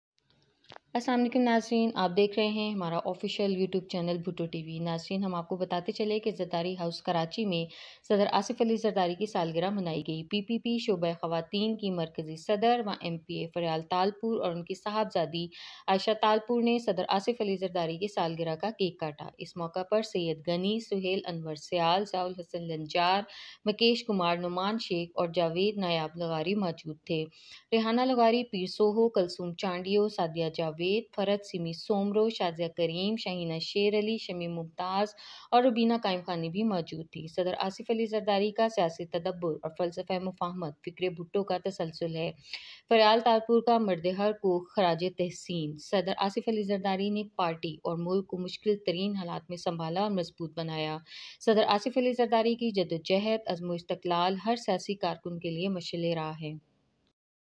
Faryal Talpur Celebrated the Birthday of King of Politics Asif Ali Zardari
سابق صدر مملکت آصف علی زرداری کی سالگرہ

زرداری ہاٶس کراچی میں صدر آصف علی زرداری کی سالگرہ مناٸی گٸی

پی پی پی شعبہ خواتین کی مرکزی صدر و ایم پی اے فریال تالپور اور ان  کی صاحبزادی عاٸشہ تالپور نے صدر آصف علی زرداری کی سالگرہ کا کیک کاٹا

اس موقعے پر سعید غنی، سہیل انور سیال، ضیإ الحسن لنجار، مکیش کمار، نعمان شیخ اور جاوید نایاب لغاری موجود تھے

ریحانہ لغاری، ہیر سوہو، کلثوم چانڈیو، سعدیہ جاوید، فرحت سیمی سومرو، شازیہ کریم، شاہینہ شیر علی، شمیم ممتاز اور روبینہ قاٸمخانی بھی موجود تھیں

 صدر آصف علی زرداری کا سیاسی تدبر اور فلسفہ مفاہمت فکرِ بھٹو کا تسلسل ہے: فریال تالپور کا مردِ حر کو خراج تحسین

صدر آصف علی زرداری نے پارٹی اور ملک کو مشکل ترین حالات میں سنبھالا اور مضبوط بنایا: فریال تالپور

صدر آصف علی زرداری کی جدوجہد، عزم و استقلال ہر سیاسی کارکن کے لیے مشعل راہ ہیں: فریال تالپور